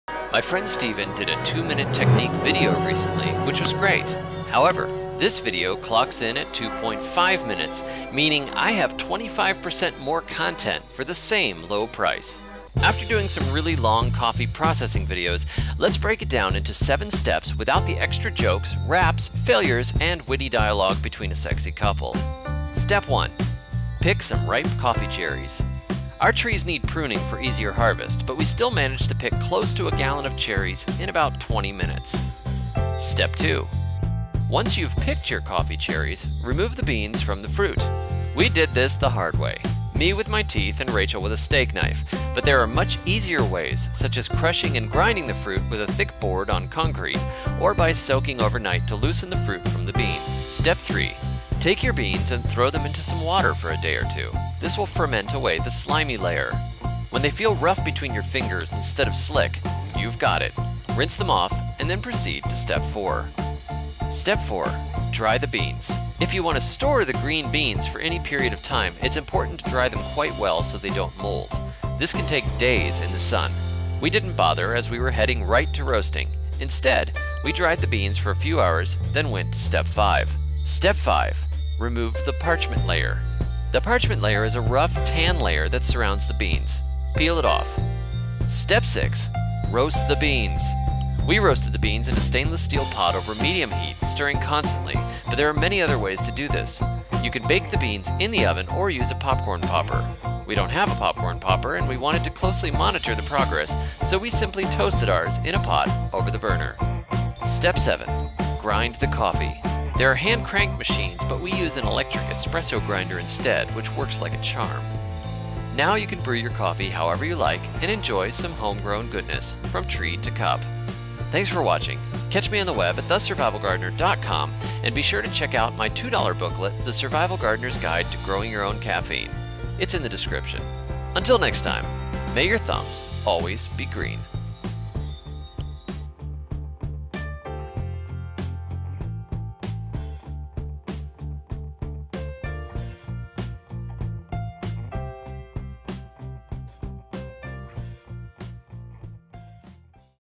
How to Process Coffee at Home in 7 Steps (2.5 Minute Version)
Processing coffee at home isn't as tough as you might think. It takes some time, sure, but it also makes for GREAT coffee!

After doing some really long coffee processing videos, let’s break it down into seven steps without the extra jokes, raps, failures and witty dialog between a sexy couple.

Step 1: Pick some ripe coffee cherries! Our trees need pruning for easier harvest but we still managed to pick close to a gallon of cherries in about 20 minutes.

Step 2: Once you’ve picked your coffee cherries, remove the beans from the fruit. We did this the hard way – me with my teeth and Rachel with a steak knife, but there are much easier ways, such as crushing and grinding the fruit with a thick board on concrete or by soaking overnight to loosen the fruit from the bean.

Step 3: Take your beans and throw them into some water for a day or two. This will ferment away the slimy layer on the beans. When they feel rough between your fingers instead of slick, you’ve got it. Rinse them off and then proceed to step four.

Step 4: Dry the beans. If you want to store the green beans for any period of time, it’s important to dry them quite well so they don’t mold. This can take days in the sun. We didn’t bother as we were heading right to roasting. Instead, we dried the beans for a few hours, then went to step 5.

Step 5: remove the parchment layer. The parchment layer is a rough, tan layer that surrounds the beans. Peel it off.

Step 6: Roast the beans. We roasted the beans in a stainless steel pot over medium heat, stirring constantly, but there are many other ways to do this. You can bake the beans in the oven or use a popcorn popper. We don’t have a popcorn popper and we wanted to closely monitor the progress so we simply toasted ours in a pot over the burner.

Step 7: Grind the coffee

There are hand-cranked machines but we used a little electric espresso grinder instead which works like a charm.

Now you can brew your coffee however you like and enjoy some homegrown goodness, from tree to cup.

Until next time, may your thumbs always be green.